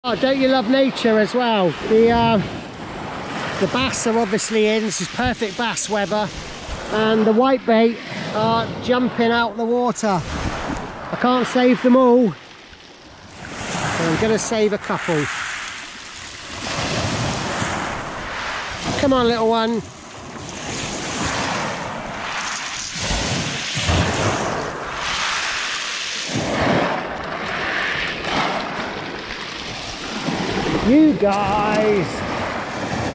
Whitebait beaching themselves to avoid the Bass. Natural phenomenon every year in Devon
Every year along our beautiful coastline, the massive shoals of Whitebait come and with them the predatory fish come too.
As the bass and Mackerel decimate the shoals, 100's of the fleeing fish fling themselves on shore, I could not possibly save them all but i manged to send a good few handfuls back into the surf.